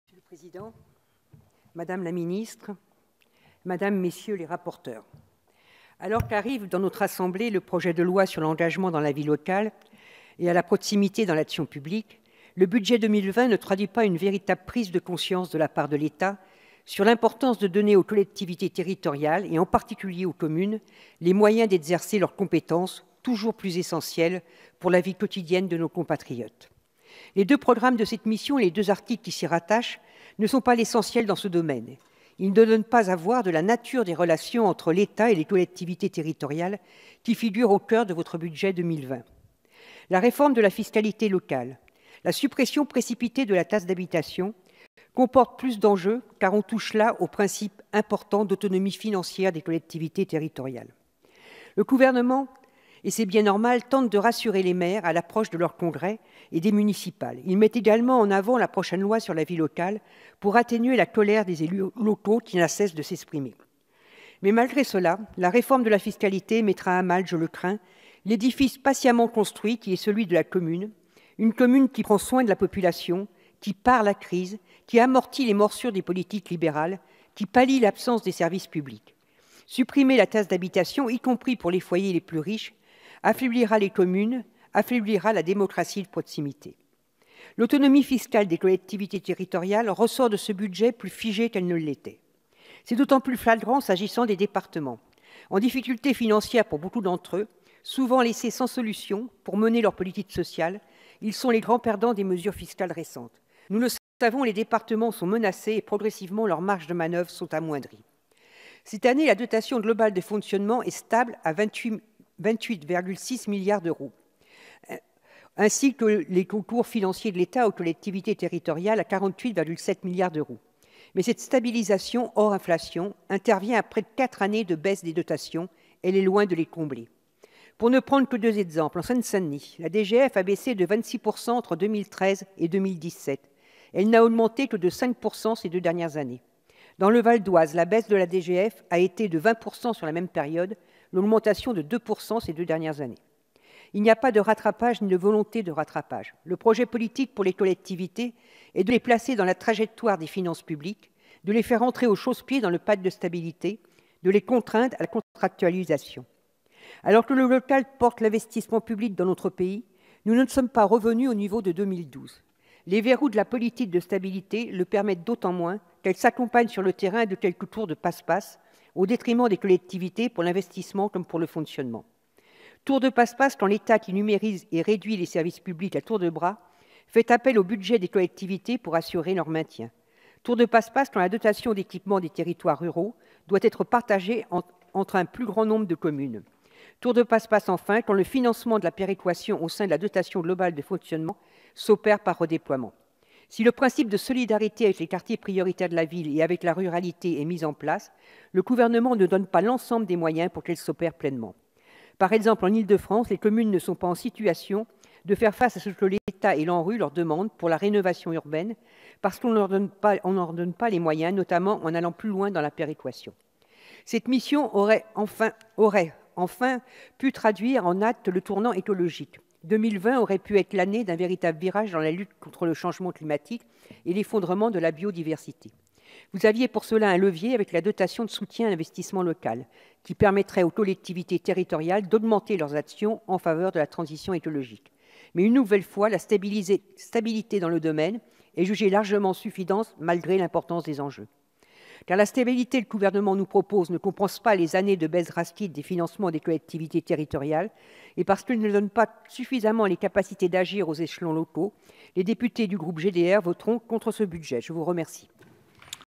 Relations collectivités territoriales - Marie-George Buffet
"Cette mission aurait pu traduire en acte le tournant écologique ; 2020 aurait pu être l'année d'un véritable virage dans la lutte contre le changement climatique et l'effondrement de la biodiversité. Vous aviez pour cela un levier avec le soutien à l'investissement local, ce qui permettrait aux collectivités territoriales d'augmenter leurs actions en faveur de la transition écologique. Enfin, le projet que le gouvernement nous propose ne compense pas les années de baisses drastiques du financement des collectivités territoriales, parce-qu'il ne donne pas suffisamment la possibilité d'agir aux échelons locaux, les députés du groupe GDR voteront contre ce budget."